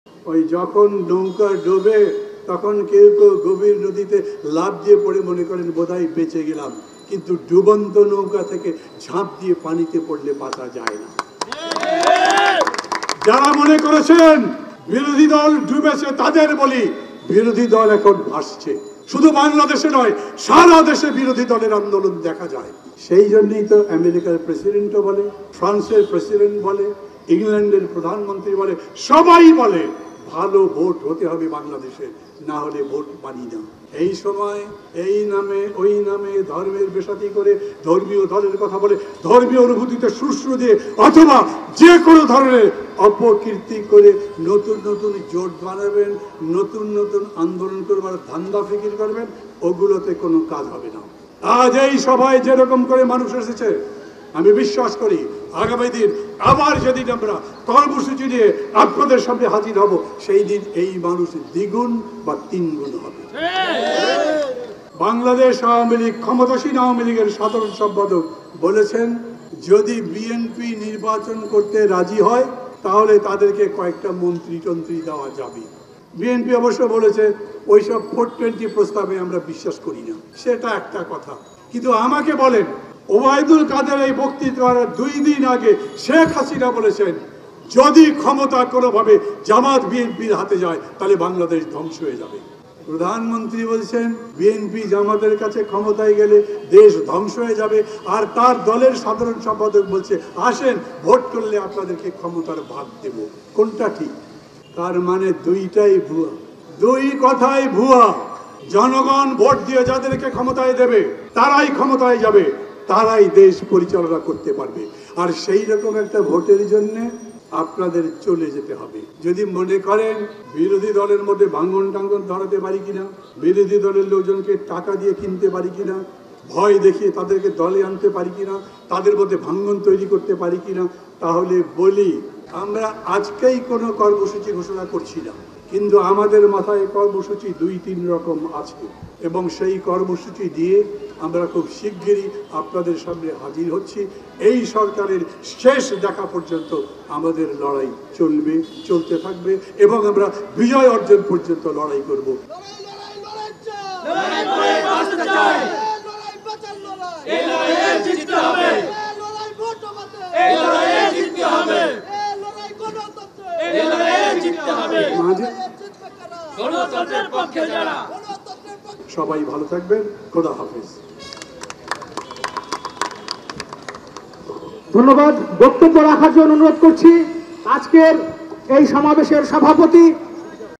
হঠাৎ বিএনপিকে এই প্রস্তাবের মানে কি ? সমাবেশেই নৌকাকে ডুবন্ত দেখিয়ে দিলেন মান্না ,দেখুন
ডুবন্ত নৌকা থেকে ঝাঁপ দিয়ে বাঁচা যায় না ! প্রধানমন্ত্রীর উদ্দেশ্যে মান্না